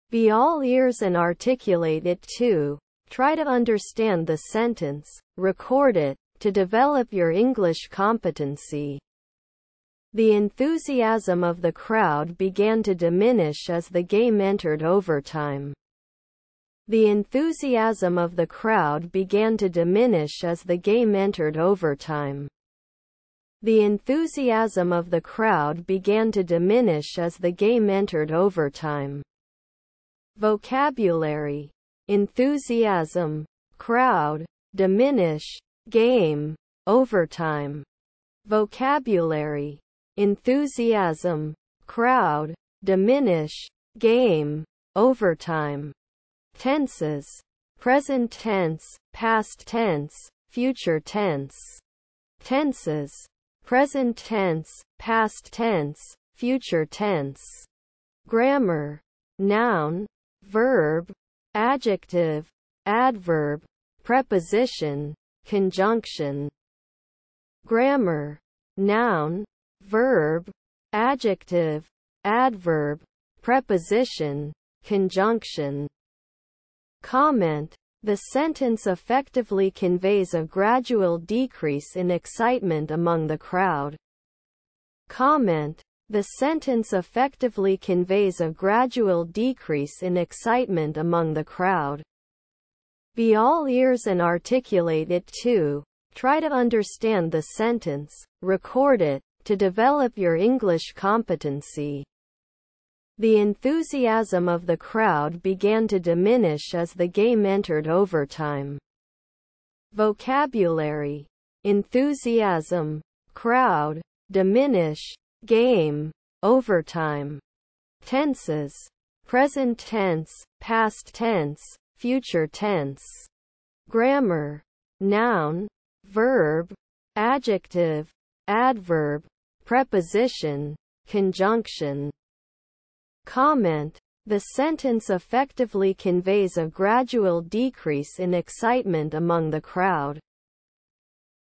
A Project Manager oversees and coordinates various aspects of a project to ensure its success.
Focus and repeat after me to comprehend the sentence and develop your English fluency.

Vocabulary: Project Manager, oversees, coordinates, aspects, success.
Tenses: Present simple, present continuous, present perfect, past simple, past continuous, past perfect, future simple.
Grammars: Nouns, verbs, adjectives, adverbs, prepositions, conjunctions, articles, pronouns, interjections.
Comment: The Project Manager plays a crucial role in the effective execution of projects.